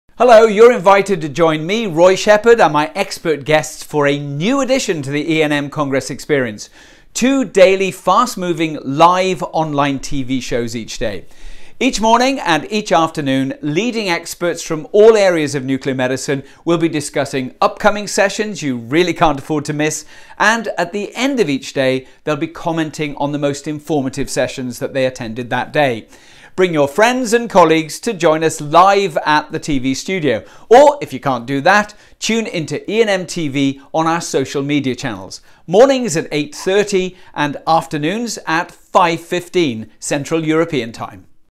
EANM'23 ONLINE TV SHOW
Get ready for an innovative addition to this year's EANM Congress experience: our brand-new EANM TV show!
Thanks to our esteemed hosts Roy Sheppard and Kate Sturgess and our featuring expert guests, you will be updated on the latest breakthroughs within the EANM community.
Gather your colleagues, and don't miss the chance to join us LIVE at the TV studio (Entrance Hall). Can't make it in person? Catch us every morning at 8:30 CEST and every afternoon at 17:15 CEST.